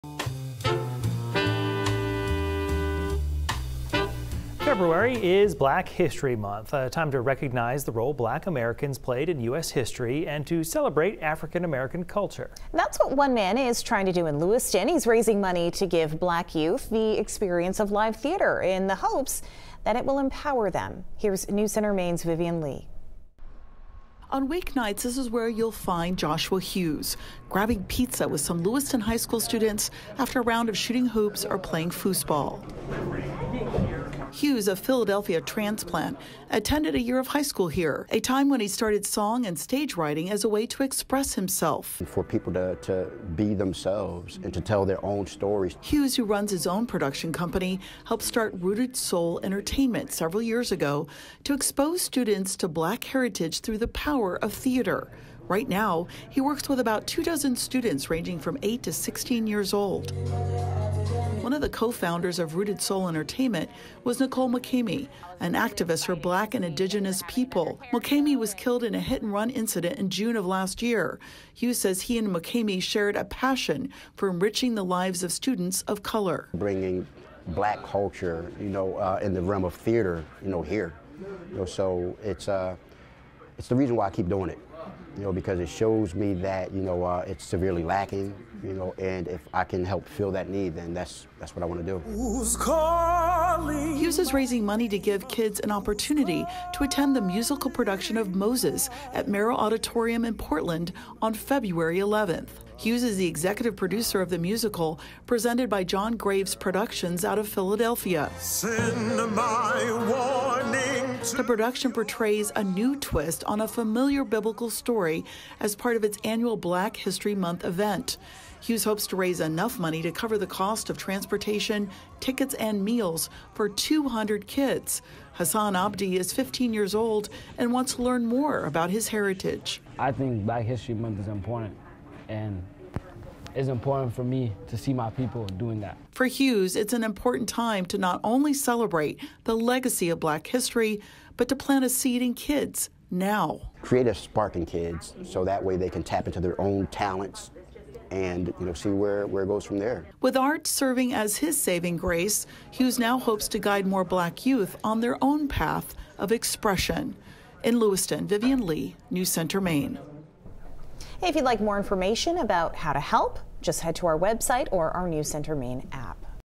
Lewiston man raises money to give Black youth the experience of live theater
February is Black History Month, a time to recognize the role Black Americans play in U.S. history and celebrate the African-American culture.